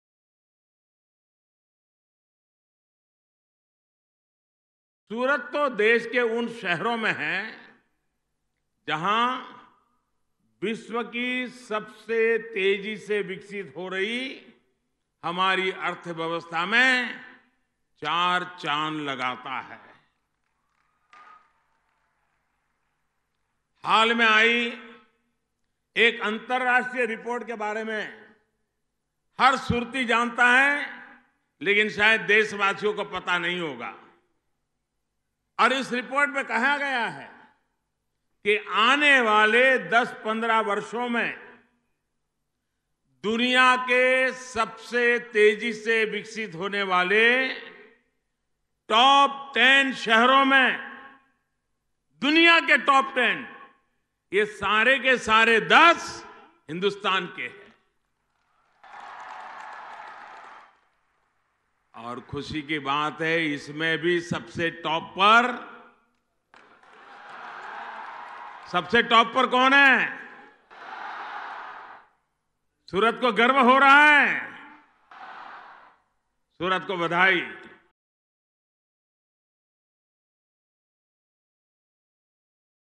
Surat will top the list of developing cities from India: PM Modi
Surat (Gujarat), Jan 30 (ANI): Prime Minister Narendra Modi after laying foundation stones of external terminal building at Surat Airport said, “Recently an international report stated that in the next 10-15 years, the top-10 fastest developing cities will be from India, and Surat will top the list of those ten cities”.